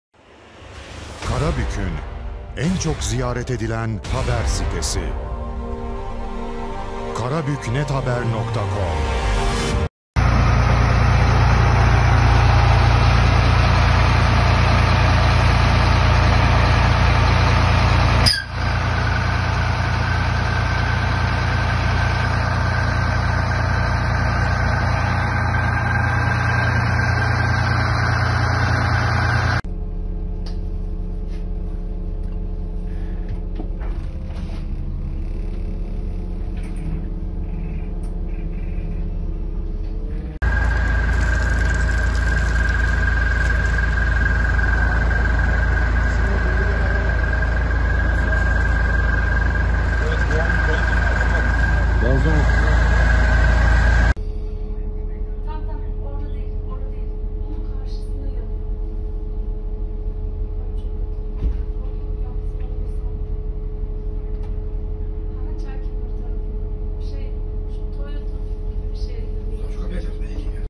SEYİR HALİNDE BİR OTOMOBİL DAHA ALEV ALDI
Karabük’ün Kayabaşı Mahallesi mevkiinde bir otomobil daha seyir halindeyken motor kısmından alev alan otomobil kullanılamaz hale geldi.

Olay, Kayabaşı köprüsünde meydana geldi. Otomobil seyir halindeyken motor kısmı bir anda alev almaya başlayan otomobil Karabük belediyesi itfaiye ekiplerince söndürüldü. O anlar cep telefonu kamerası ile kaydedildi. Ekiplerin müdahalesiyle söndürülen yangında otomobil kullanılamaz hale geldi.

Son günlerde sık yaşanan araç yangınları dikkat çekerken yangının neden çıktığı henüz bilinmiyor.